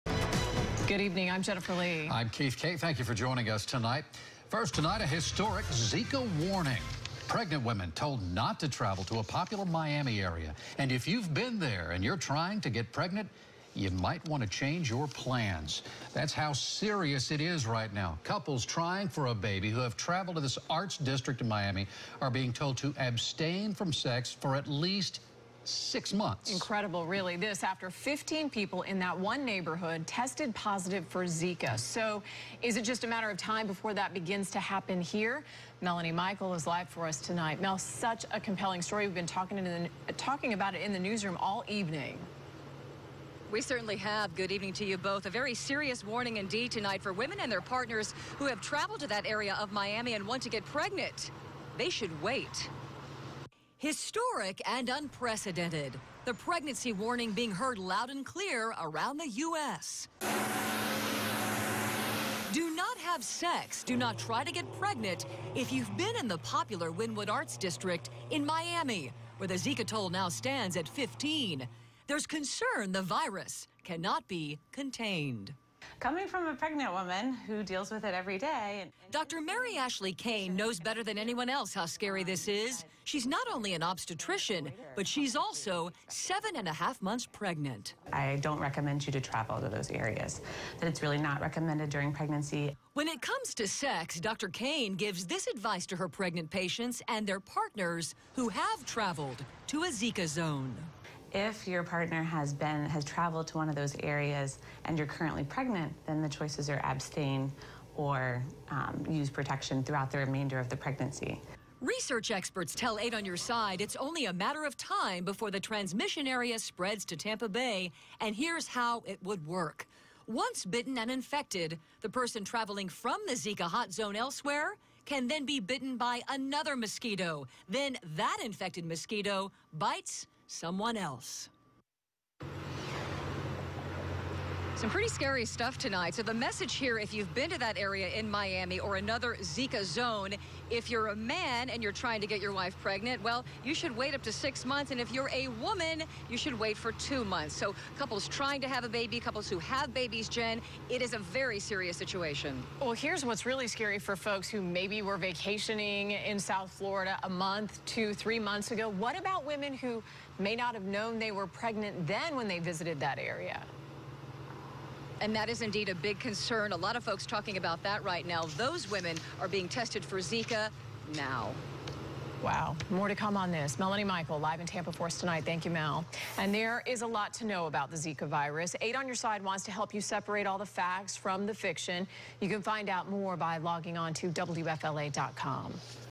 Officials advise pregnant woman to avoid area in Miami where Zika has been found
Fifteen cases of the virus were contracted in the arts district, Wynwood.